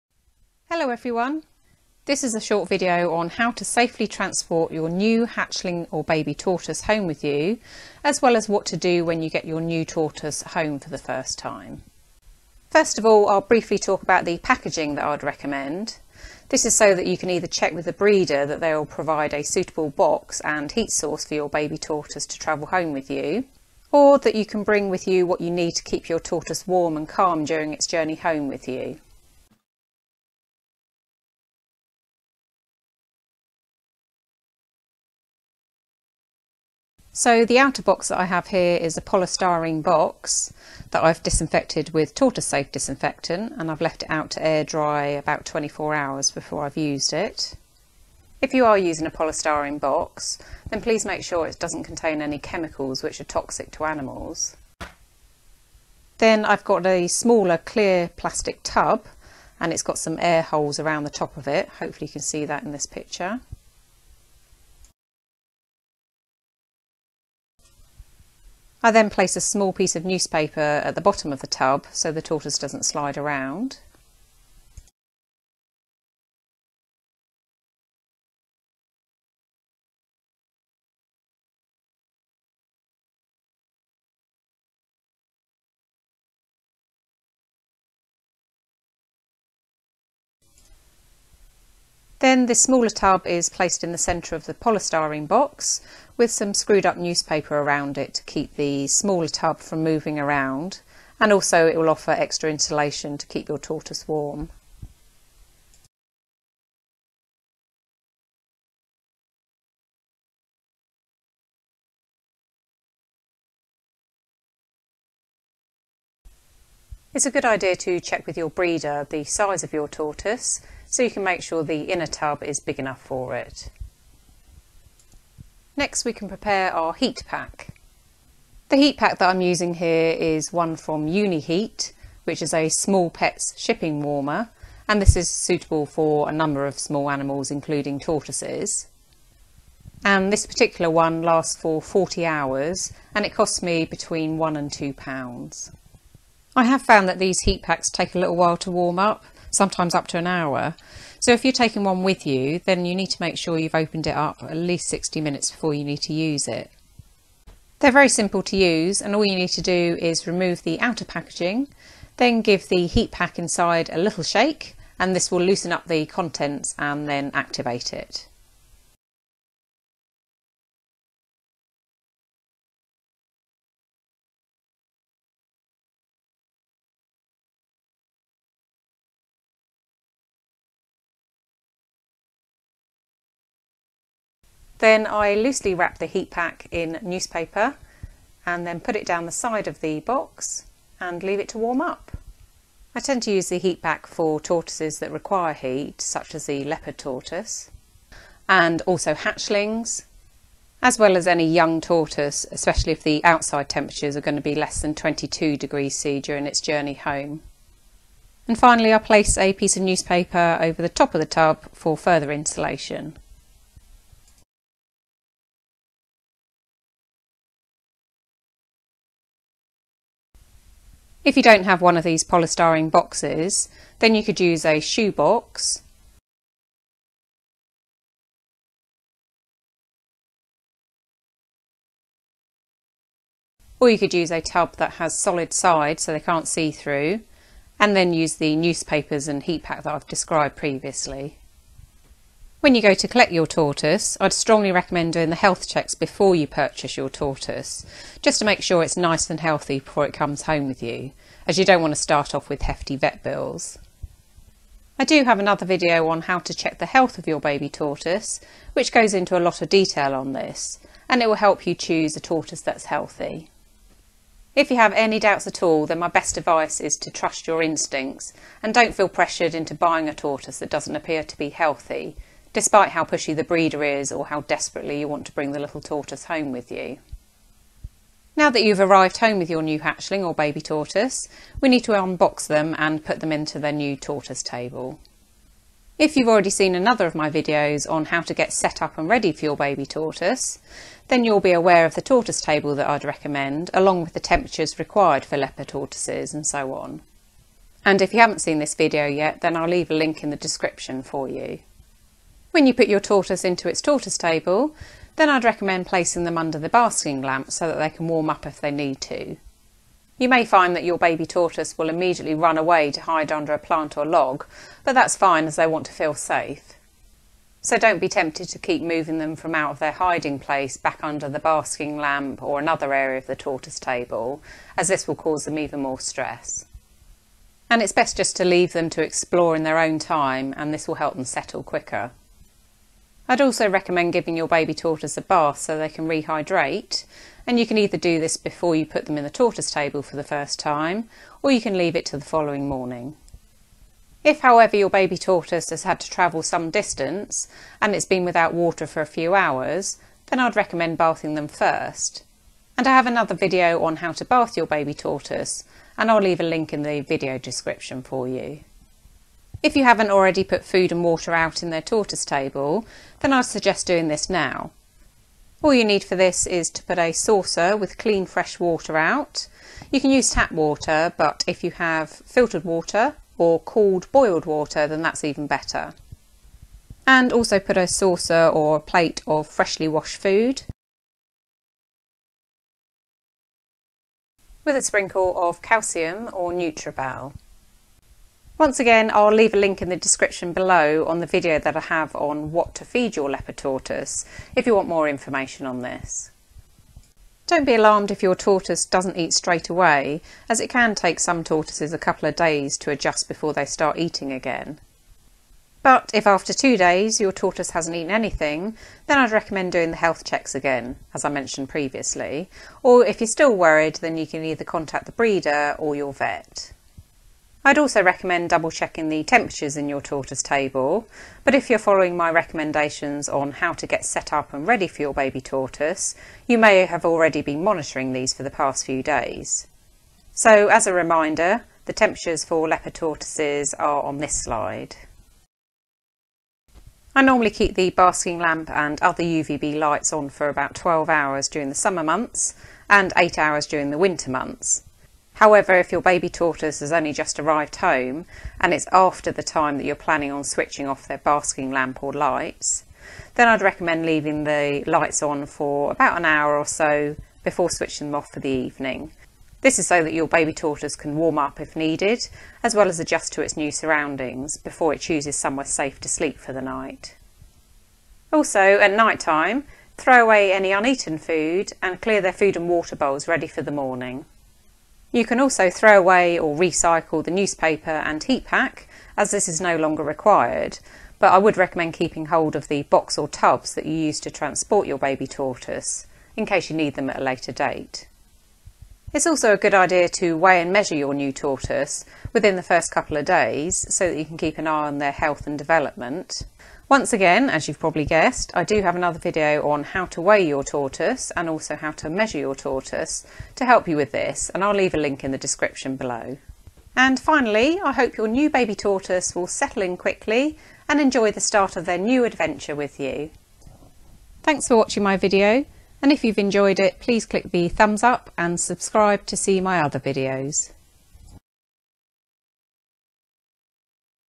Bringing your baby tortoise home | happytortoises
This is a short video on how to transport your baby tortoise home with you as well as what you can do to help your tortoise settle into it's new home.